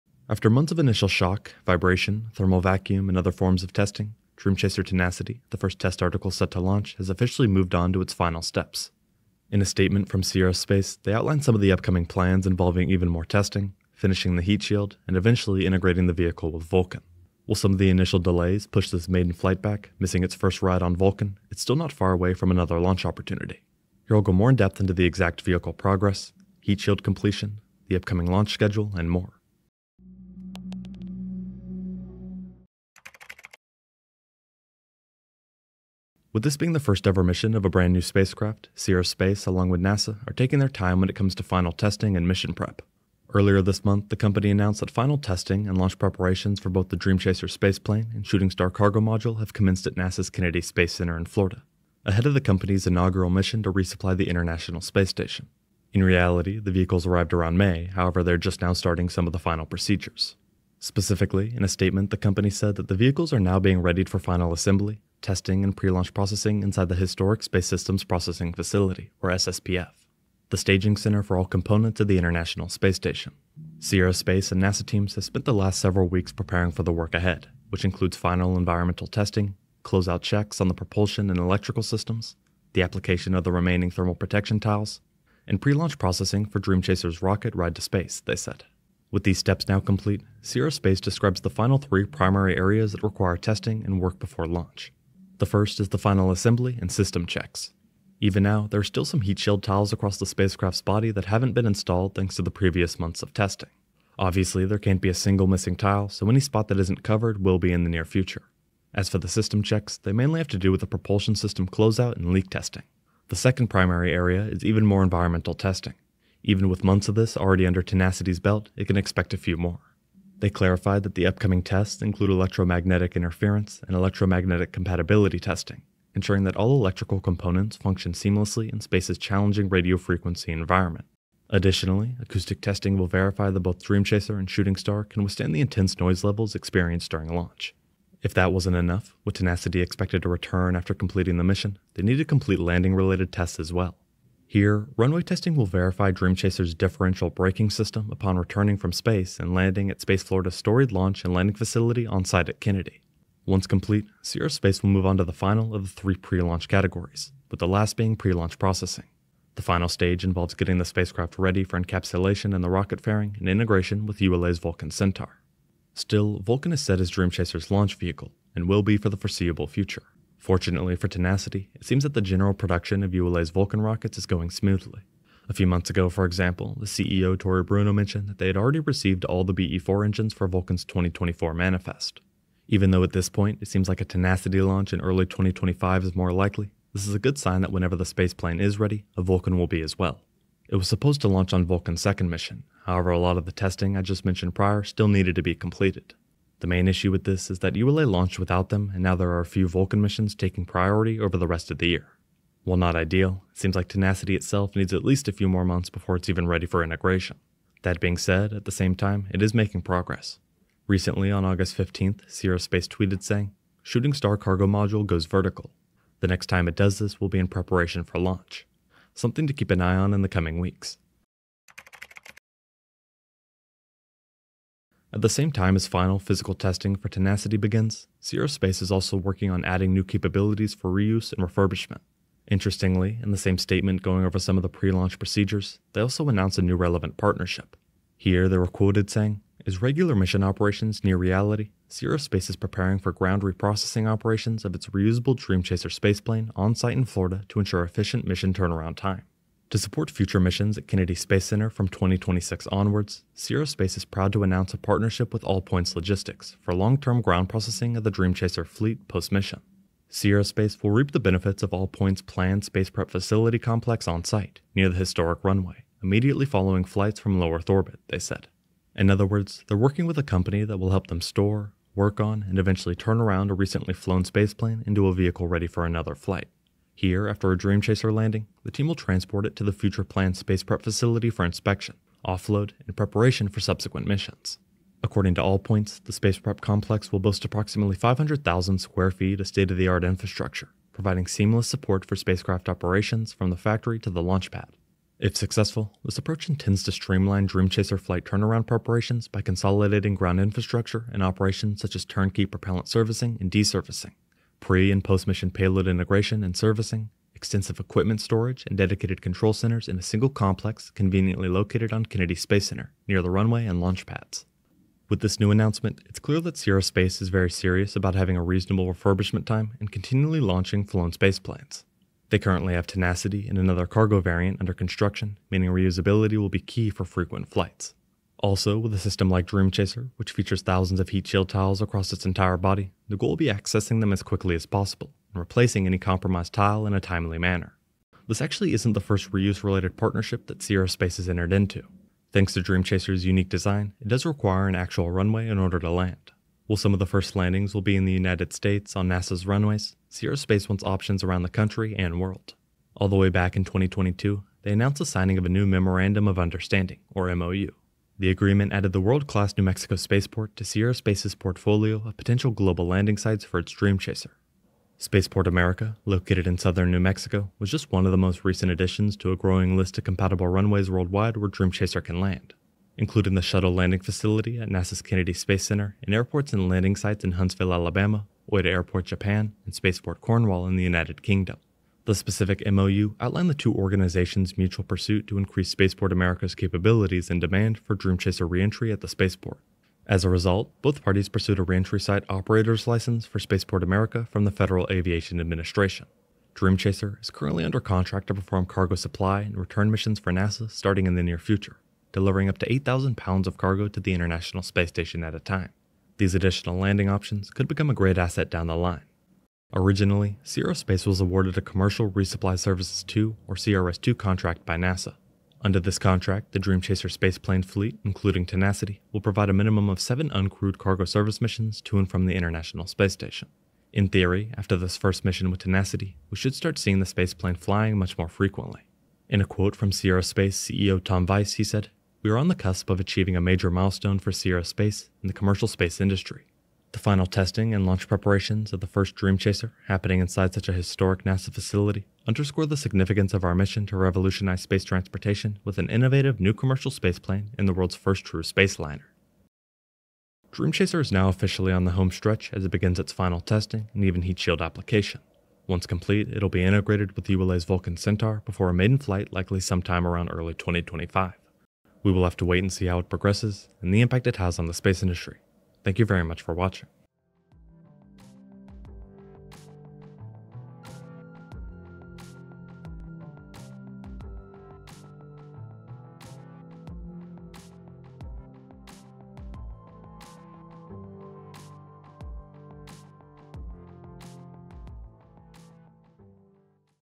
Dream Chaser Tenacity Has Officially Begun Final Testing
After months of initial shock, vibration, thermal vacuum, and other forms of testing, Dream Chaser Tenacity, the first test article set to launch, has officially moved on to its final steps. In an official statement from Sierra Space, they outlined some of the upcoming plans involving even more testing, finishing the heat shield, and eventually integrating the vehicle with Vulcan.

While some of the initial delays pushed this maiden flight back, missing its first ride on Vulcan, it still is not far away from another launch opportunity. Here I will go more in-depth into the exact vehicle progress, heat shield completion, the upcoming launch schedule, and more.

Chapters:
0:00 - Intro
0:32 - The Road to Launch
3:41 - New Reuse Opportunities